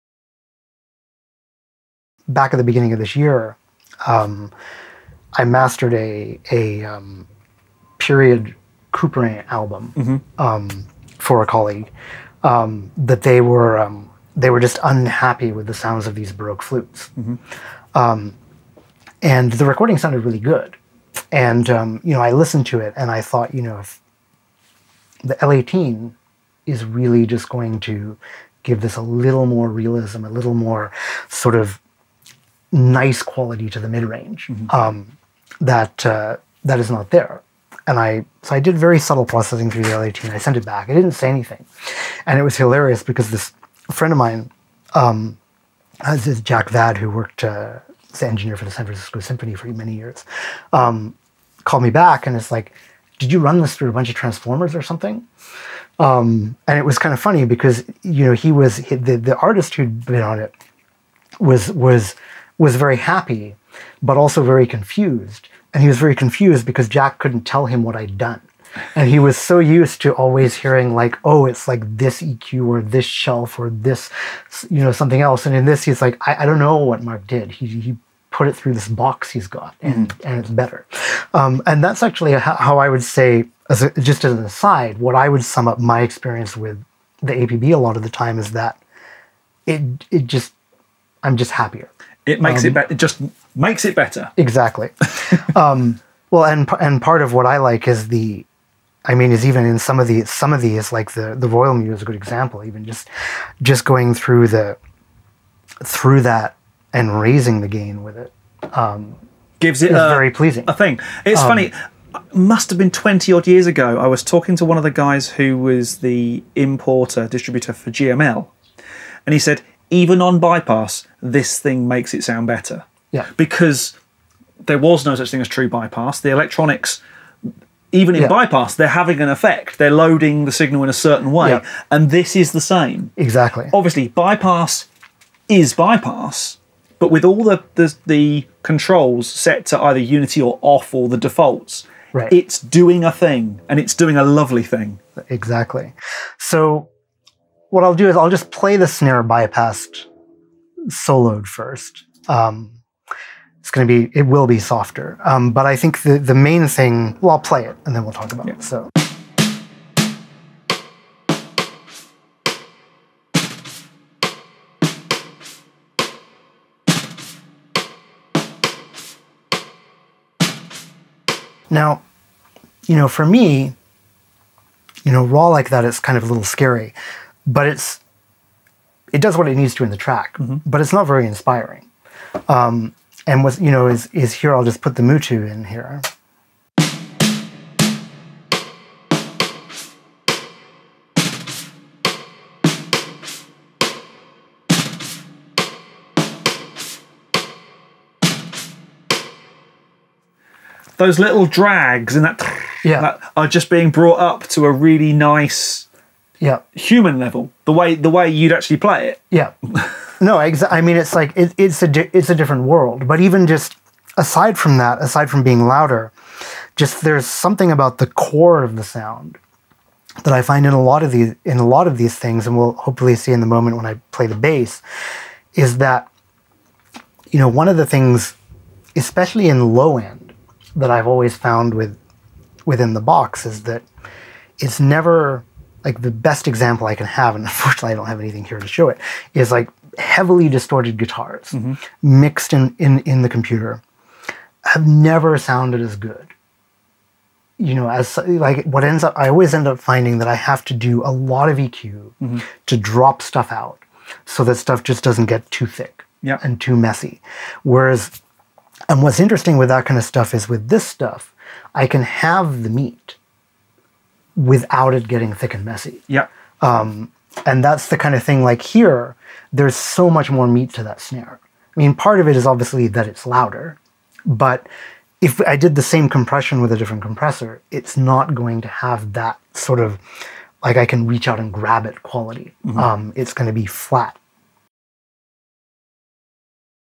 Mark Willsher (Scoring Mixer, Engineer, Editor) on the APB!
This episode of APB Artist Sessions features Mark Willsher (Scoring Mixer, Engineer, and Supervising Sound Editor for Lord of the Rings and The Hobbit Trilogy) talking about the APB, and what it adds to the sound by just being a part of the signal chain!

Let us know what you think about the APB in the comments below!

The McDSP Analog Processing Box (APB) combines the flexibility of software with the fidelity of premium analog processing. Each channel can be controlled by an APB plug-in giving true digital workflow with genuine analog performance.